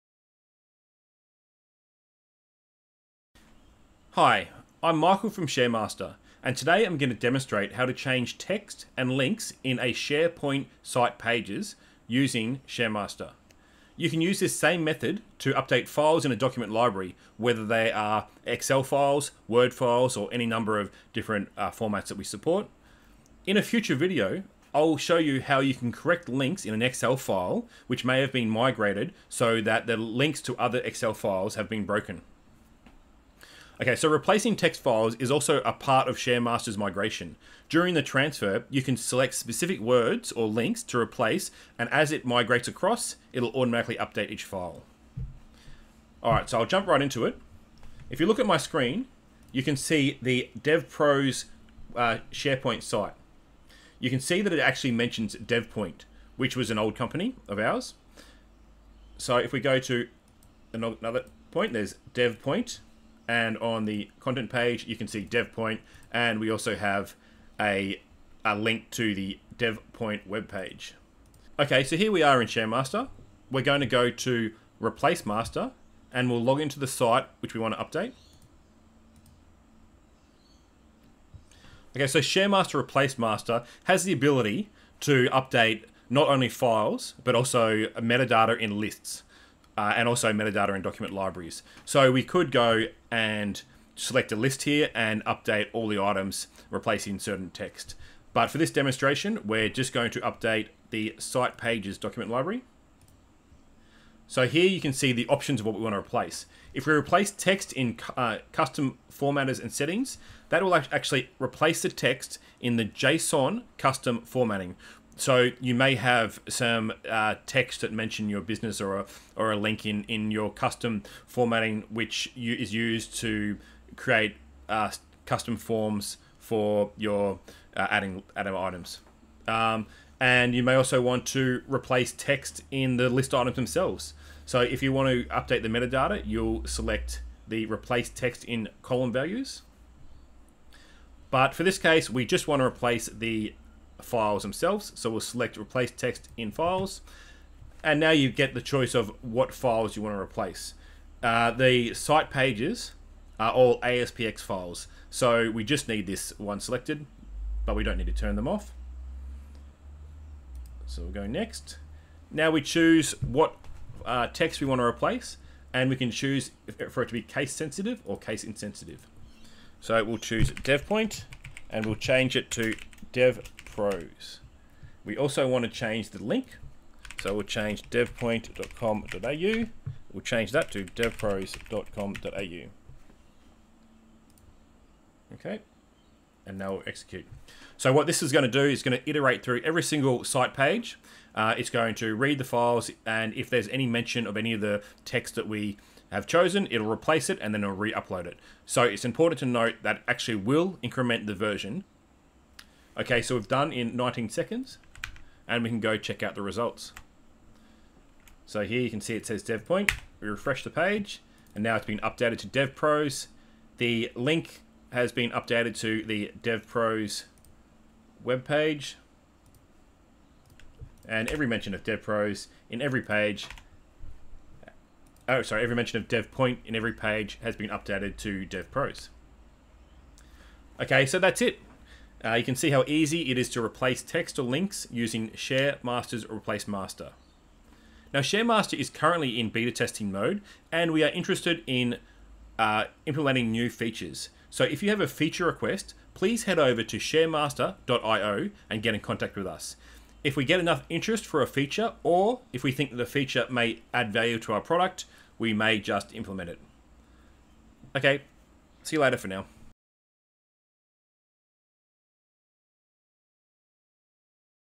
Easily replace text or links in a SharePoint Site Pages library
In this video I will demonstrate how to update text and links by bulk in a SharePoint Site Pages document library.
This same method can be used to update files, columns and JSON custom formatting in a SharePoint site.